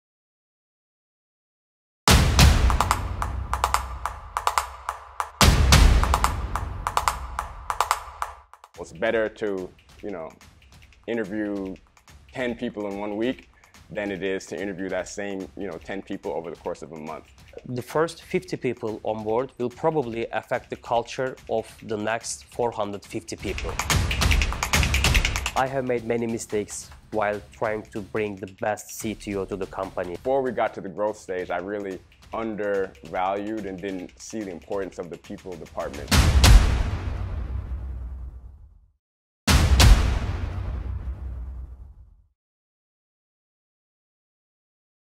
Human Capital Trailer
Learn how to implement scalable hiring practices and how to hire the right people at the right time to fuel rapid growth.